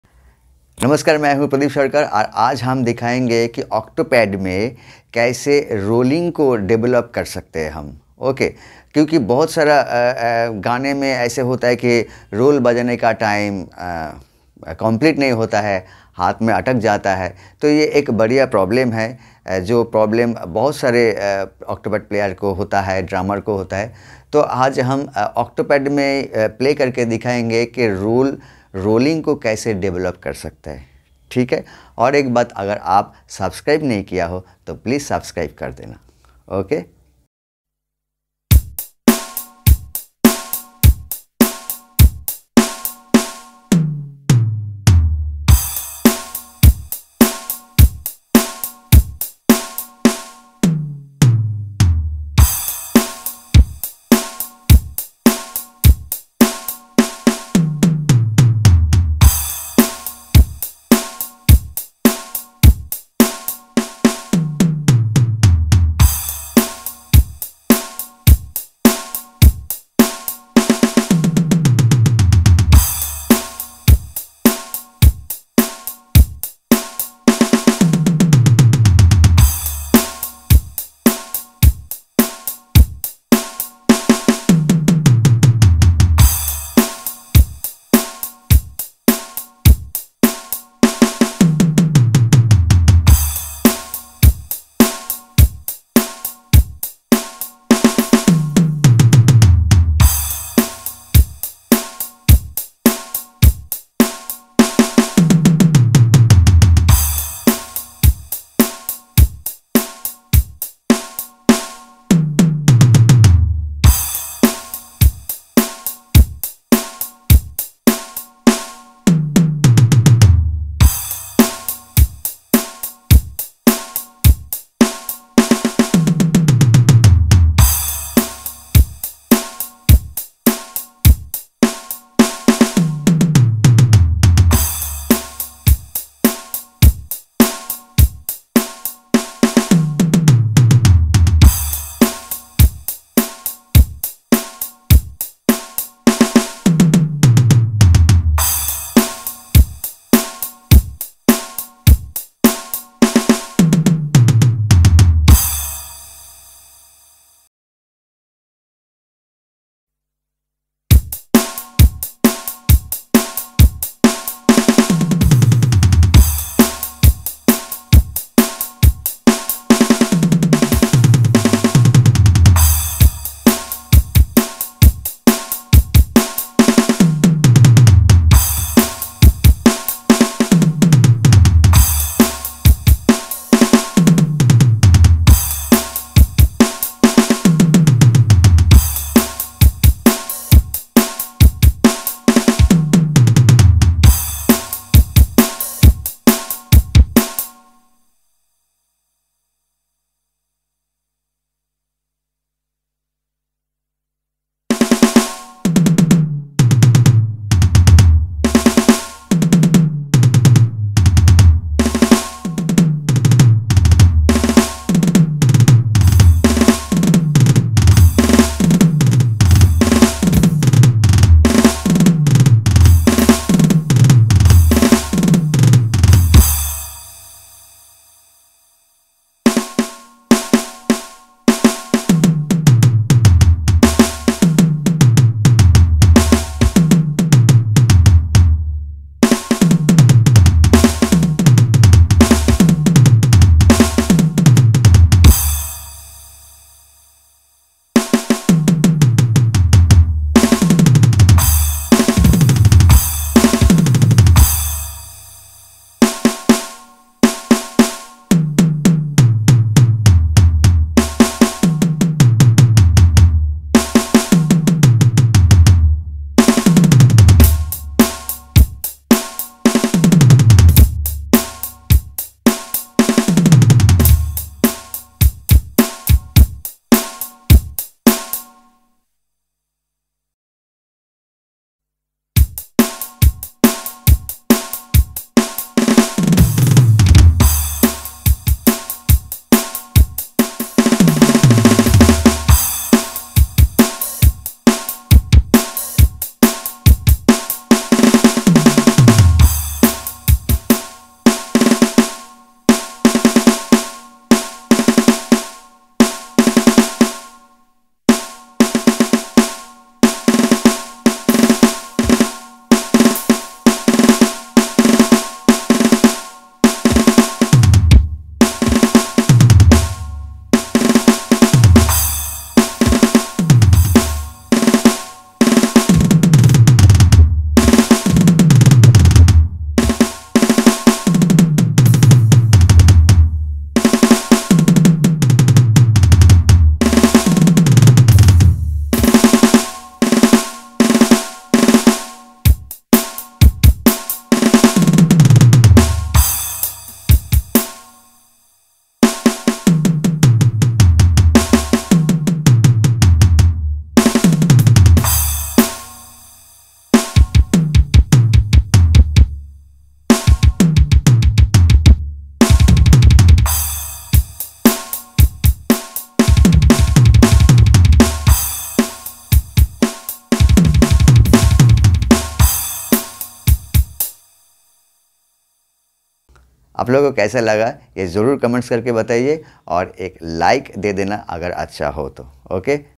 Octapad Lesson || Rolling problem ? Just Follow 4 Stapes
Hope you like this lesson
You have to practice 2 hrs daily for better results.

Pradip sarkar drummer from Kolkata